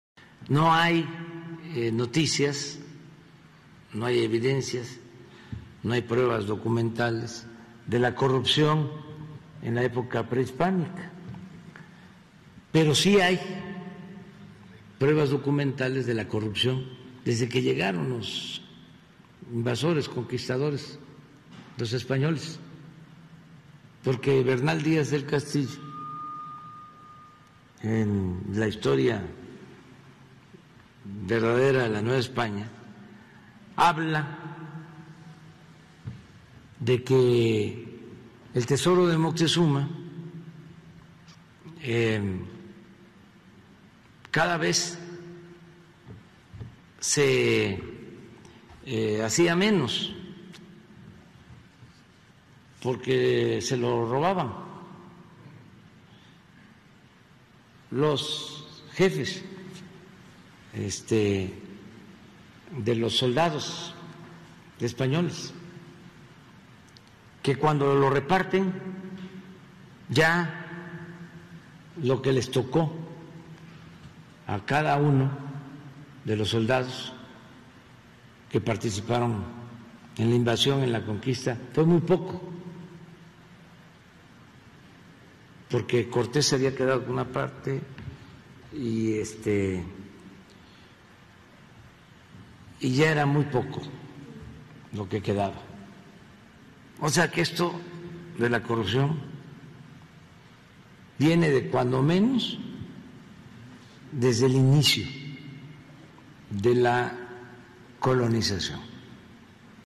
No existe registro sobre corrupción en la época prehispánica: AMLO
El presidente Andrés Manuel López Obrador aseguró que no existe registro sobre corrupción en la época prehispánica, pero que sí hay a partir de la llegada de los españoles